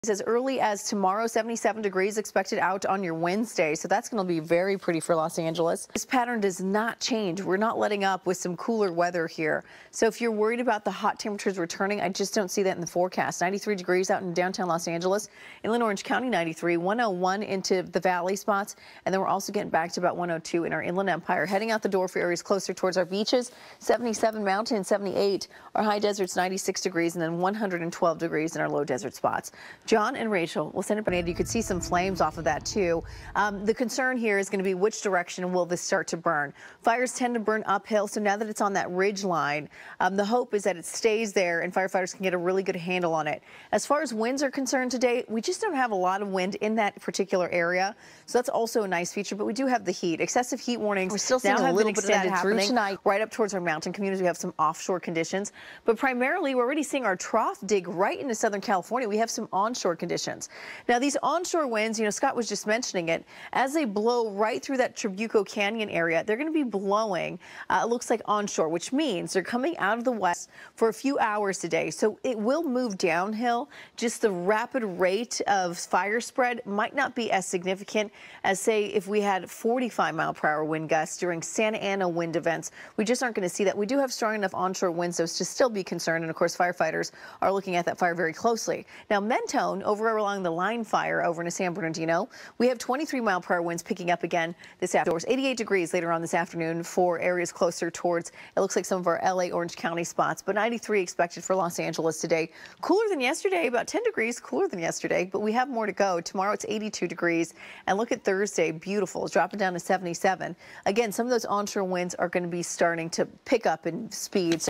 Leslie Lopez 🐐 predicts elevated fire danger
Meteorologist, Leslie Lopez explains how the weather conditions affect fires and air quality in southern California.